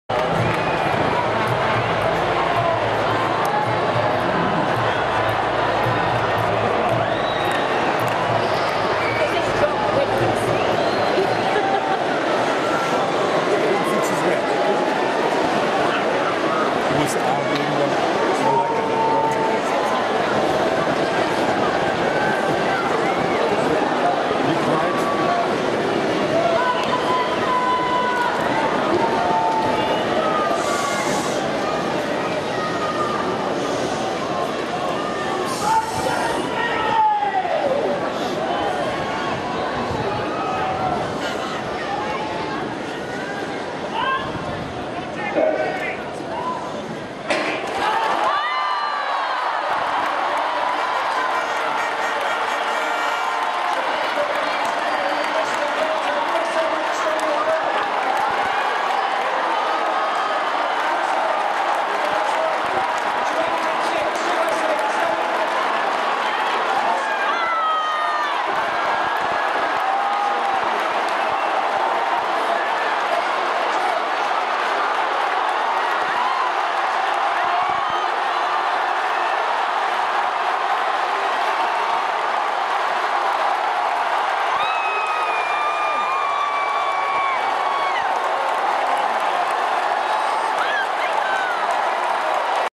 Jamaica winning the 4 x 100m relay
Jamaica winning the 4 x 100m relay and Usain Bolt does the Mo-Bot at the London 2012 Olympics